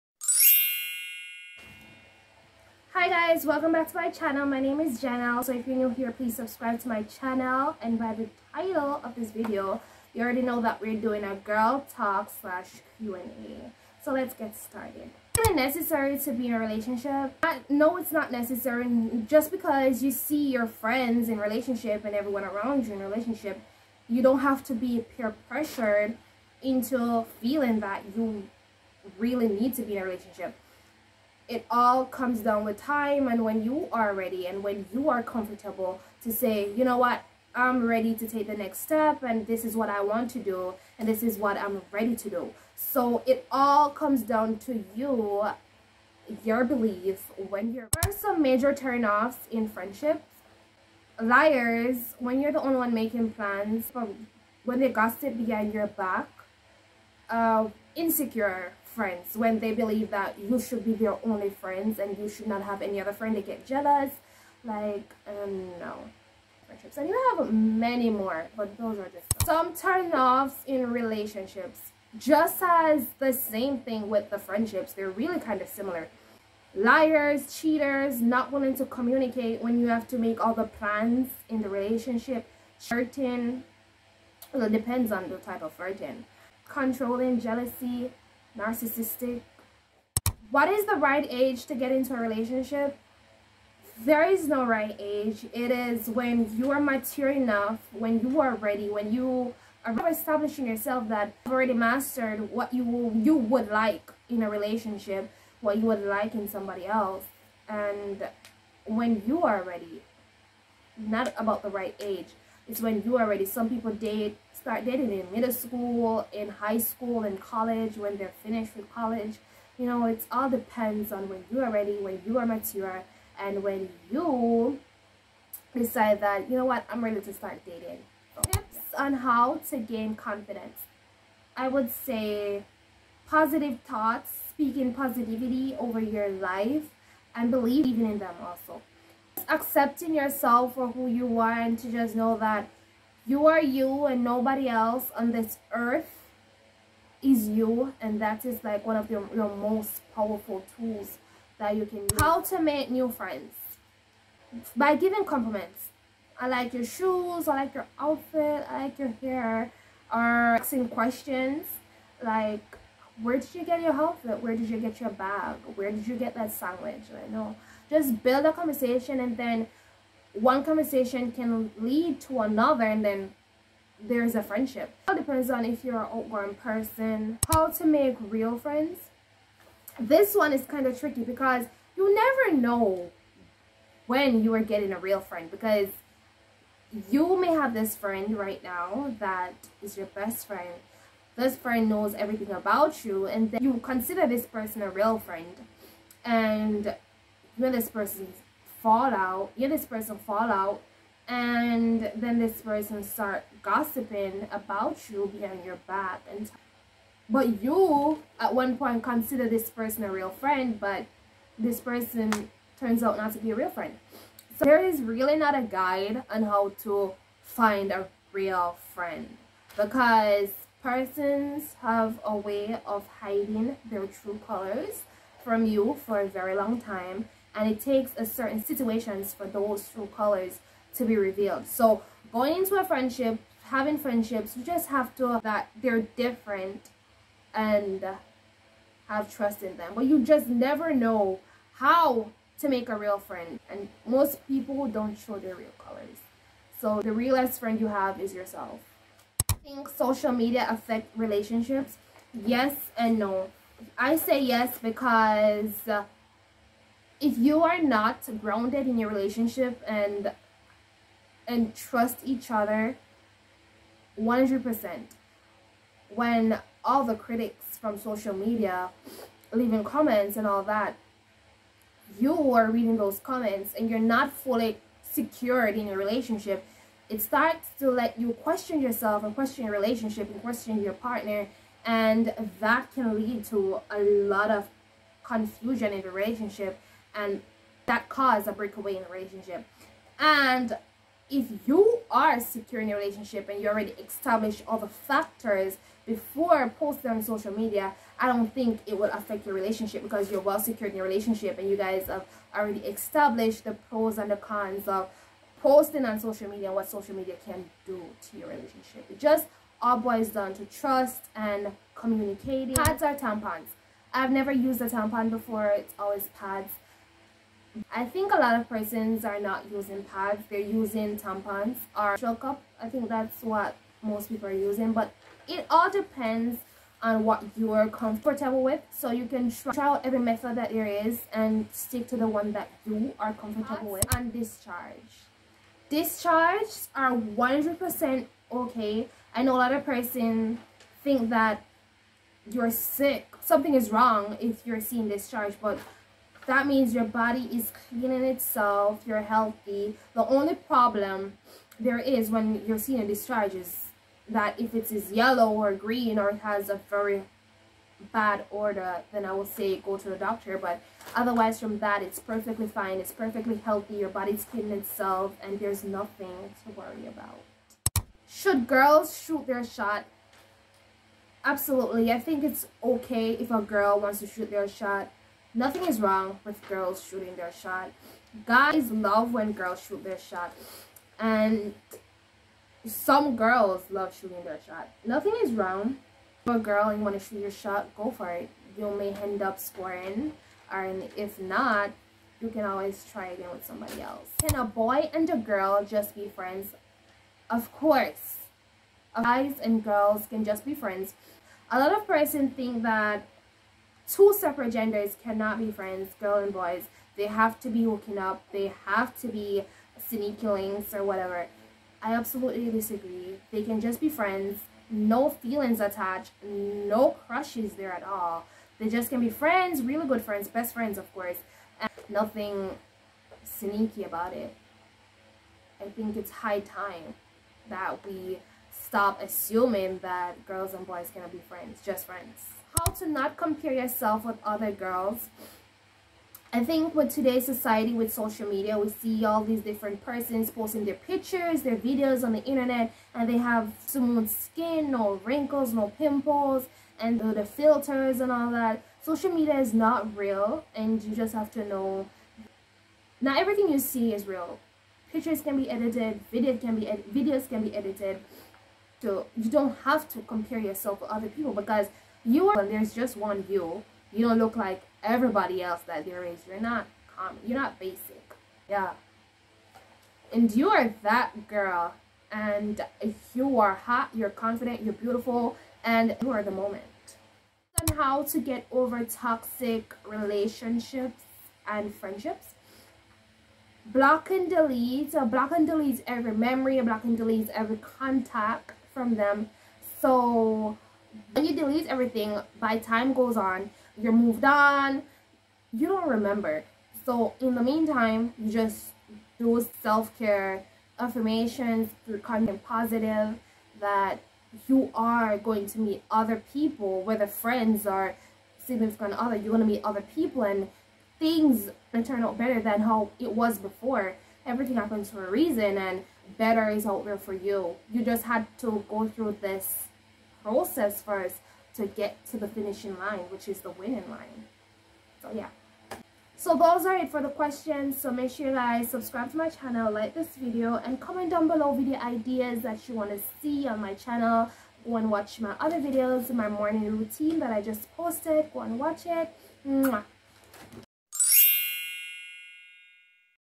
Girl Talk Q&A Friendship red flags, Confidence, Self love, Relationship
Hey guys❤ This video is all about girl talk & Q&A we talk about relationship red flags and also friendships... How to have self-love and to be confident  in your own skin...